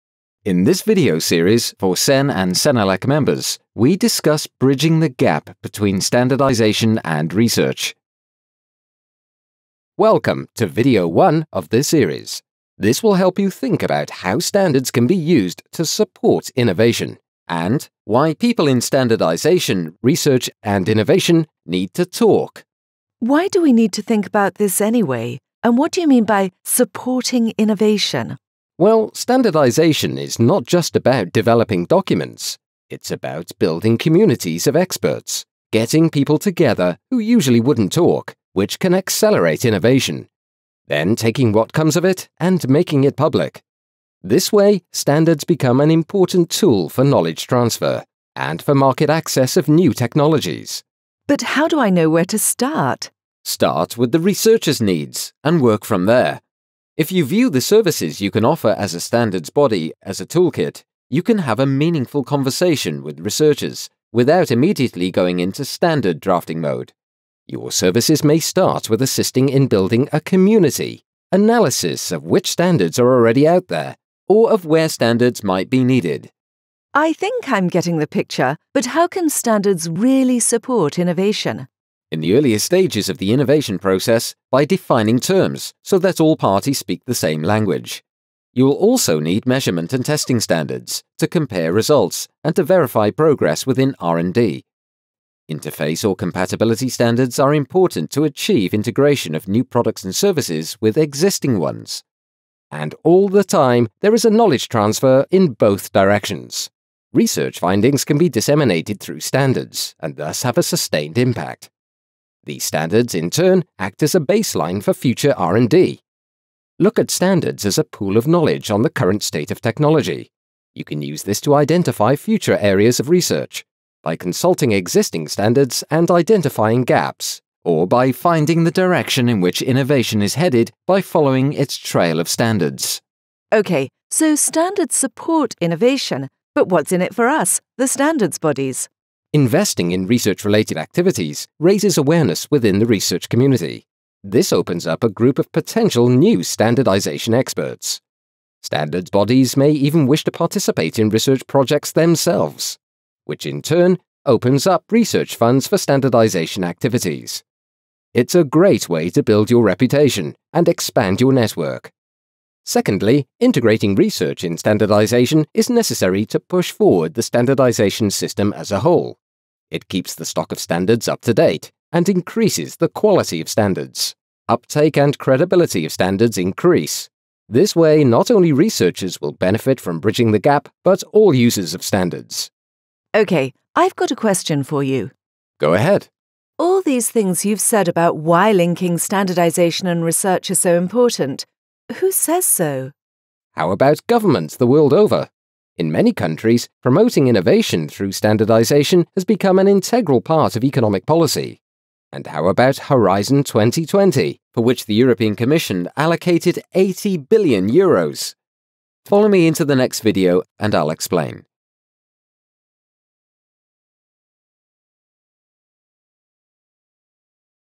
How standards support innovation - Training videos for CEN and CENELEC Members
Bridging the gap between standardization, research and innovation – training videos for the CEN and CENELEC members. This is the first video of a series:
1. How standards support innovation
2. How standards support Horizon2020
3. How to develop an internal standardization strategy to support innovation
4. How to communicate with researchers and innovators